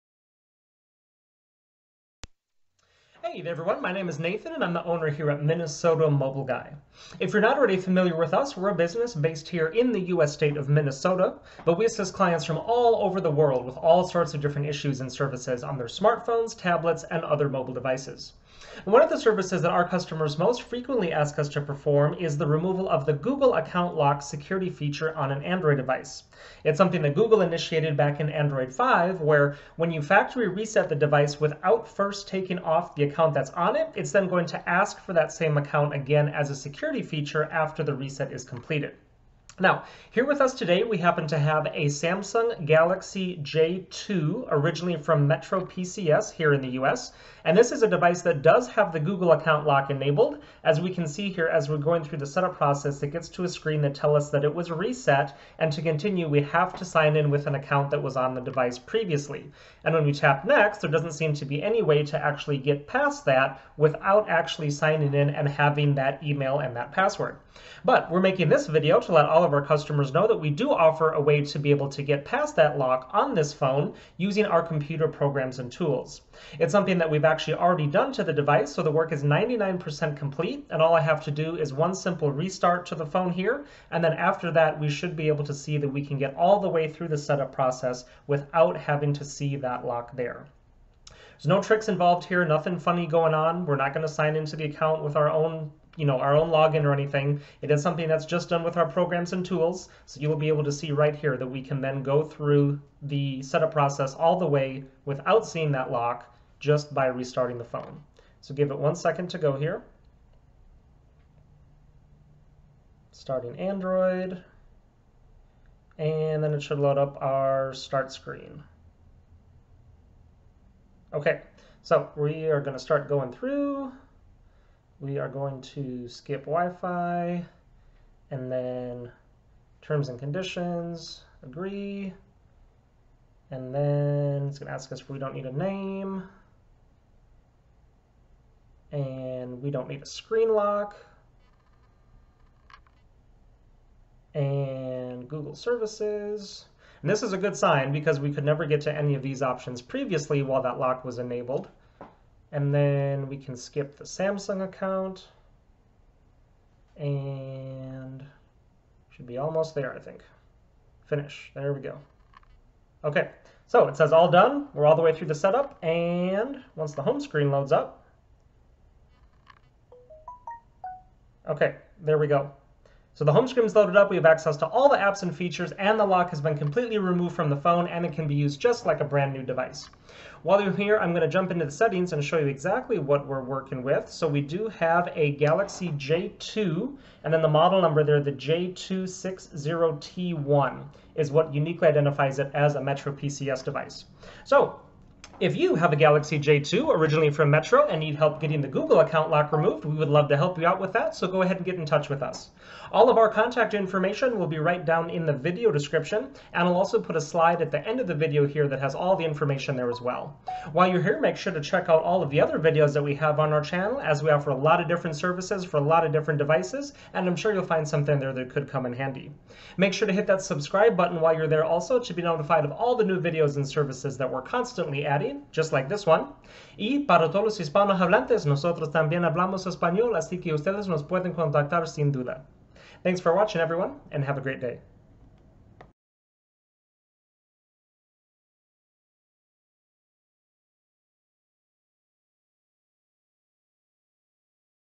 Disable Bypass Remove Google Account Lock FRP on Metro Samsung Galaxy J2!
Video showing a Google Account Lock successfully being bypassed/removed on a Metro PCS Samsung Galaxy J2, (SM-J260T1). Google includes this security feature with all devices on Android 5.1 and up so that when a device is reset without first removing the Google account, the device is locked to the previous owner's account.